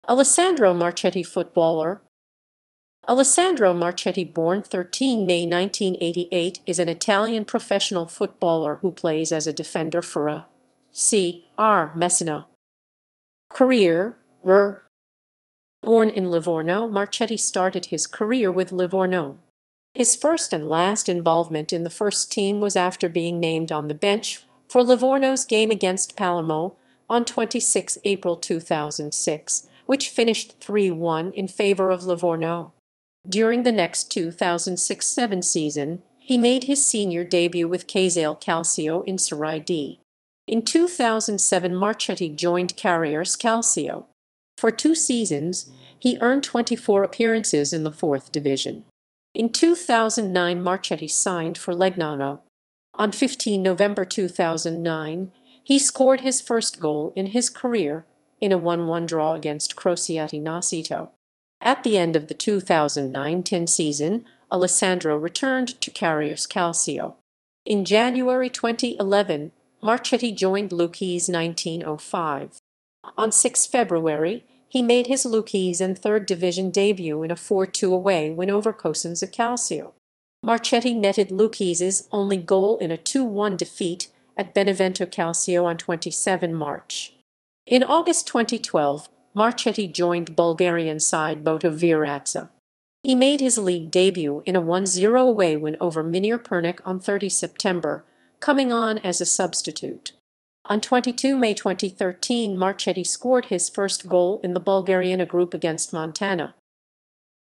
Alessandro Marchetti (footballer)
Alessandro Marchetti (born 13 May 1988) is an Italian professional footballer who plays as a defender for A.C.R. Messina.